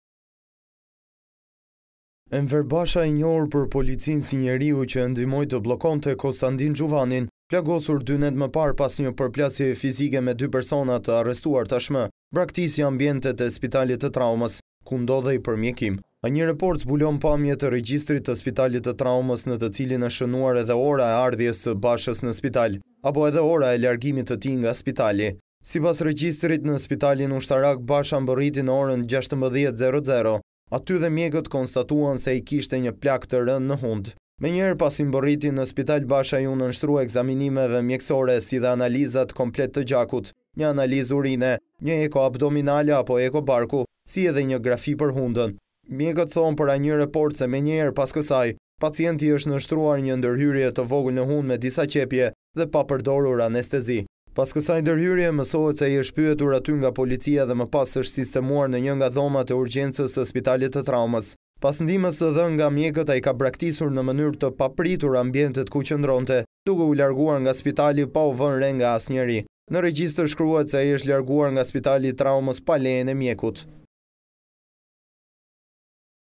A1 Report - Enver Basha braktis spitalin pa lejen e mjekut, ja dokumentet
TIRANE - Enver Basha, i njohur për policinë si njeriu që e ndihmoi të bllokonte Kostandin Xhuvanin, plagosur dy net më parë pas një përplasjeje fizike me dy persona të arrestuar tashmë, braktisi ambientet e Spitalit të Traumës, ku ndodhej për mjekim.

A1 Report zbulon pamje te regjistrit të spitalit të traumës, në të cilin është shënuar edhe ora e ardhjes së Bashës në spital, apo edhe ora e largimit të tij nga spitali.

Sipas regjistrit në spitalin ushtarak, Basha mbërriti në orën 16:00, aty dhe mjekët konstatuan se ai kishte një plagë të rëndë në hundë.

Menjëherë pasi mbërriti në spital, Basha iu nënshtrua ekzaminimeve mjekësore, si analizat komplet të gjakut, një analizë urine, një eko abdominale, apo eko barku, si dhe një grafi për hundën.

Mjekët thonë për A1Report se menjëherë pas kësaj, pacienti i është nënshtruar një ndërhyrjeje të vogël në hundë me disa qepje, dhe pa përdorur anestezi.

Pas kësaj ndërhyrjeje mësohet se ai është pyetur aty nga policia dhe më pas është sistemuar në një nga dhomat e urgjencës së spitalit të traumës.

Pas ndihmës së dhënë nga mjekët ai ka braktisur në mënyrë të papritur ambientet ku qëndronte, duke u larguar nga spitali, pa u vënë re nga njeri.

Në regjistër shkruhet se ai është larguar nga Spitali i Traumës pa lejen e mjekut.